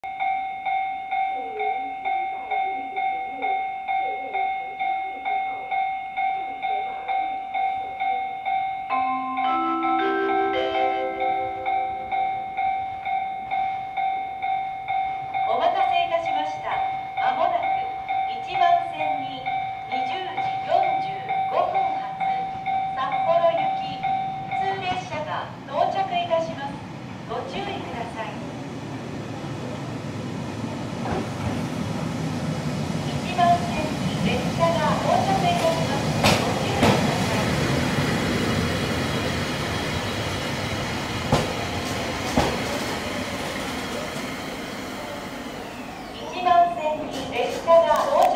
室蘭本線737系 登別駅入線
室蘭本線の737系普通列車札幌行きが登別駅に入線するシーンです。